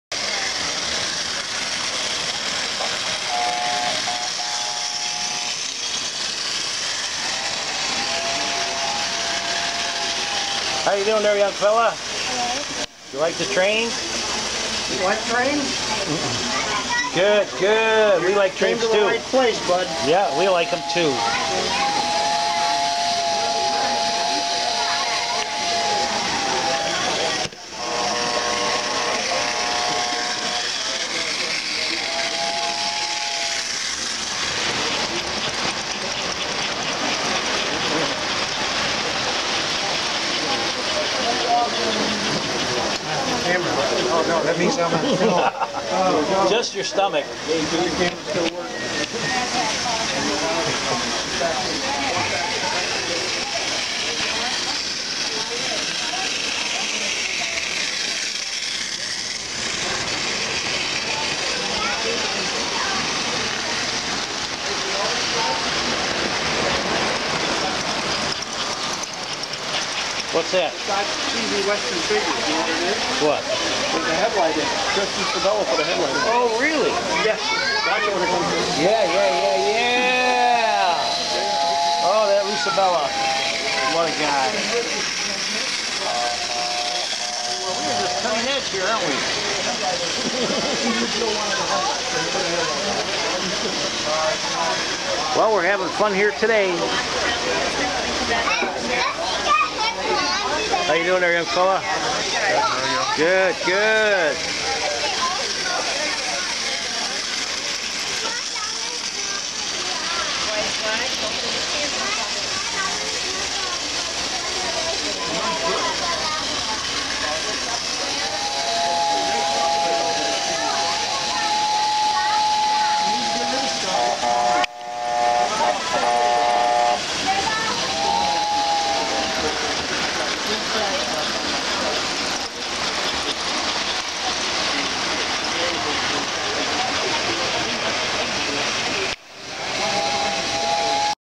SAFE Young Men & American Flyer Trains in Pinellas
The SAFE American Flyer Train Club runs trains for public enjoyment, but it is always a thrill to see young people watching our trains up close for the first time.